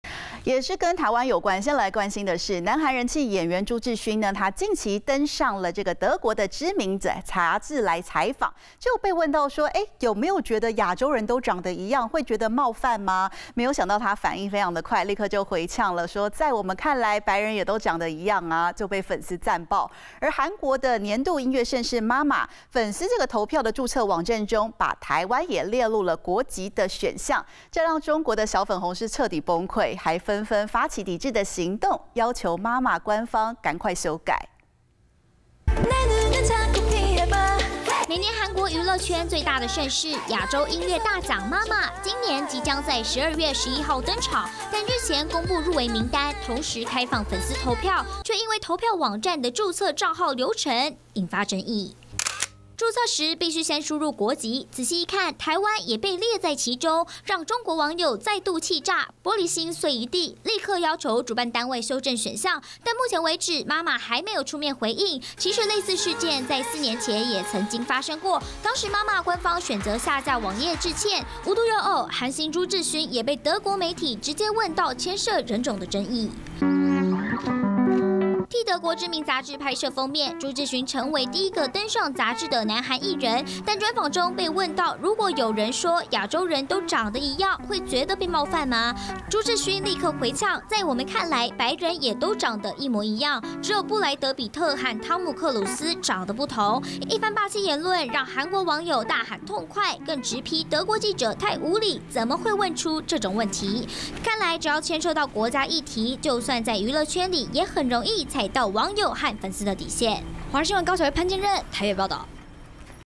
亞洲人長一樣? 韓星朱智勳回應網讚爆｜華視新聞 20211106
每年韓國娛樂圈最大的盛事，亞洲音樂大獎MAMA，今年即將在12月11日登場，但日前公布入圍名單同時開放粉絲投票，卻因為投票網站的註冊帳號流程引發爭議。註冊時必須先輸入國籍，仔細一看台灣也被列在其中，讓中國網友再度氣炸玻璃心碎一地，立刻要求主辦單位修正選項。